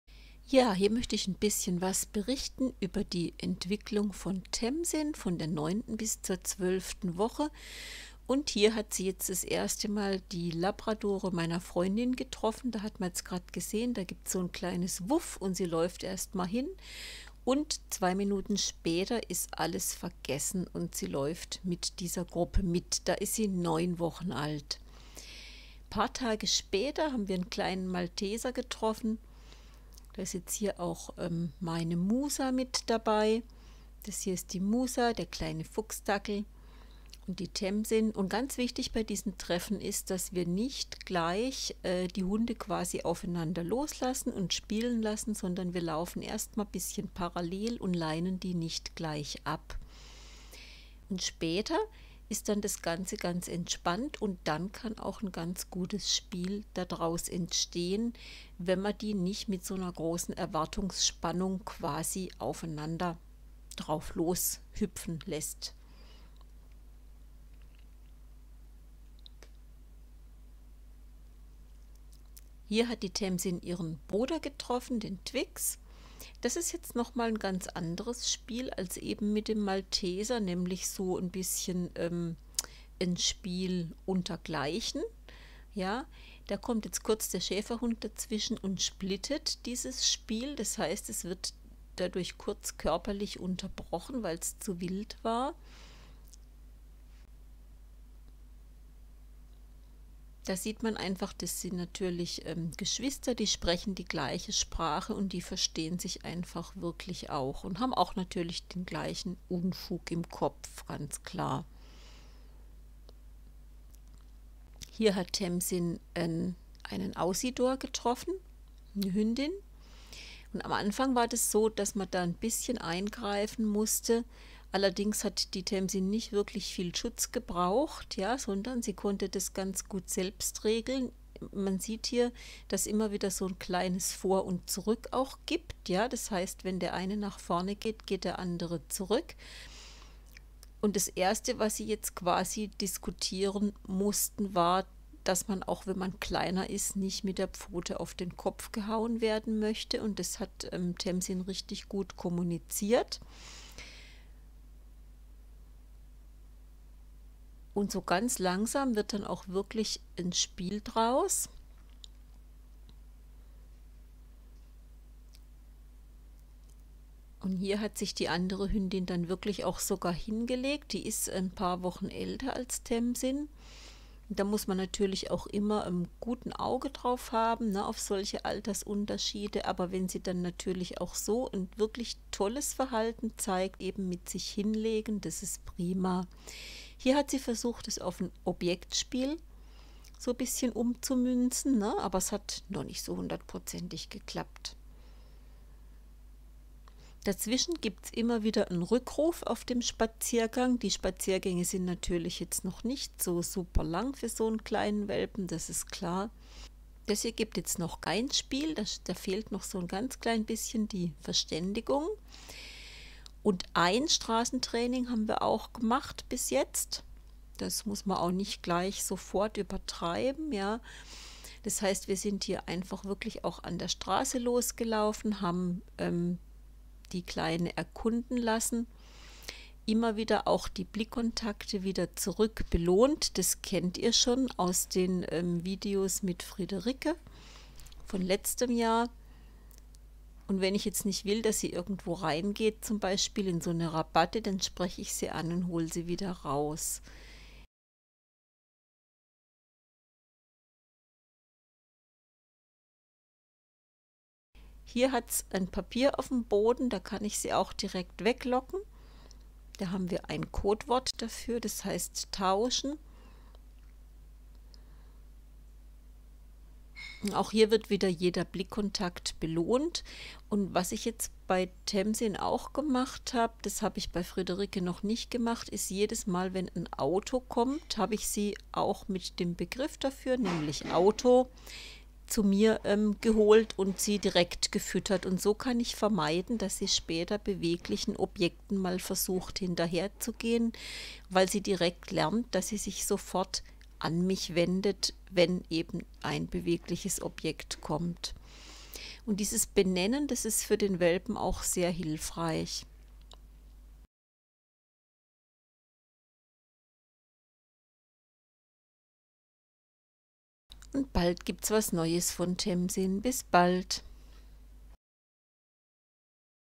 Tamzin - 9bis 12 Wochen
Hier seht Ihr ein paar Momente aus dem Leben der kleinen Tamzin, von 9 bis 12 Wochen. Tamzin trifft verschiedene Hunde und hat das erste Straßentraining.